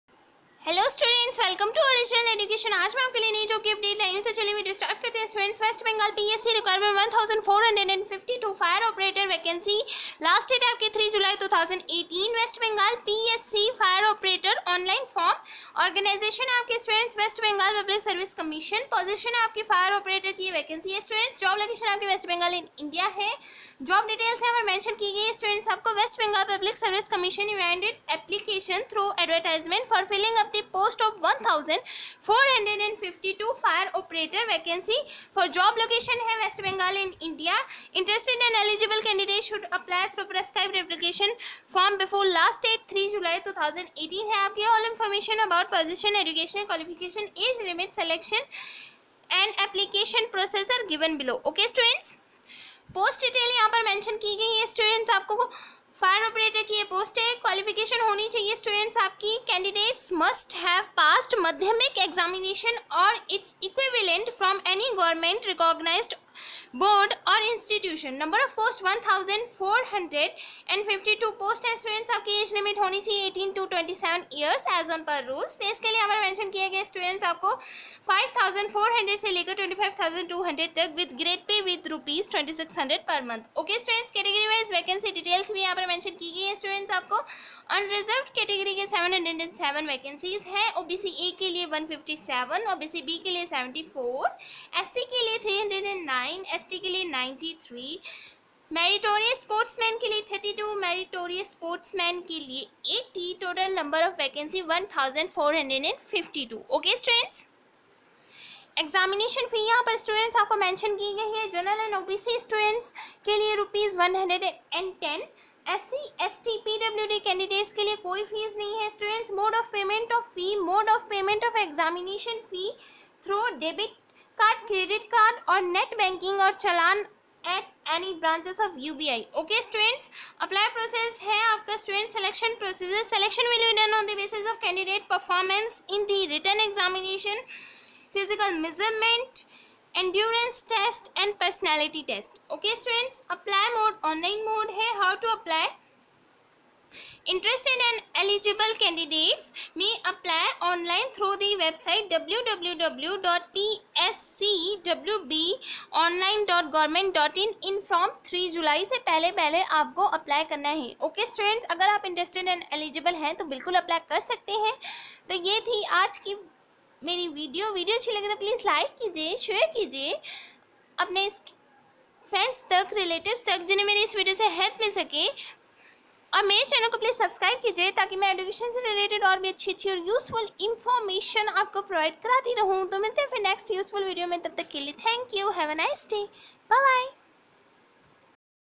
Psc Recruitment- 1452 Fire Operator Vacancy-Last Date 03 July 2018.
West Bengal PSC FIre Operator Online Form Post OF 1452 fire Operator Vacancy For Job Location West Bengal ,India.Interested and Eligible Candidate Should Apply through prescribed application form before last Date 03 July 2018.